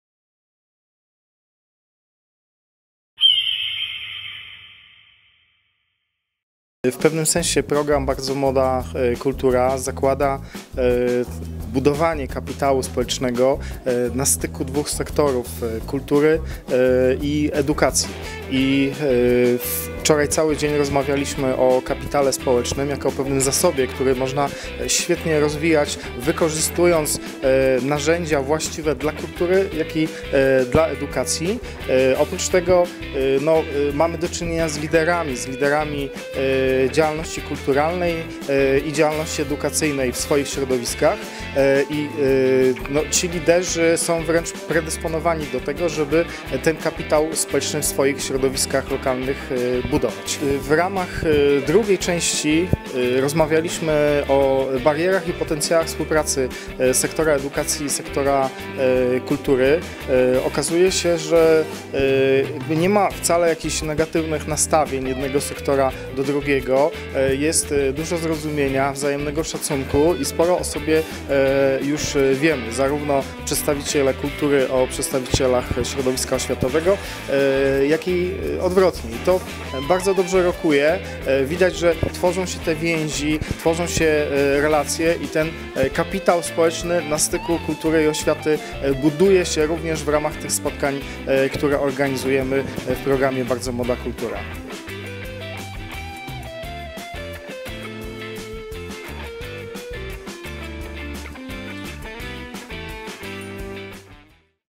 BMK 2016 - Paweł Kamiński o kapitale społecznym w projektach edukacyjnych 13,14-07-2016
W Stroniu Śląskim zakończyły się drugie warsztaty szkoleniowe organizowane w ramach projektu Bardzo Młoda Kultura na Dolnym Śląsku.

Cykl krótkich reportaży ze spotkań informacyjnych projektu BMK na DŚ zrealizowanych przez DOKIS.pl. od marca do maja 2016 r. Wideo prezentuje wypowiedzi uczestników szkolenia i jednocześnie liderów projektów regrantingowych.
Miejsce: Stronie Śląskie.
Realizacja: Agencja Publicystyczna DOKIS.pl, wg koncepcji M. Borowskiego.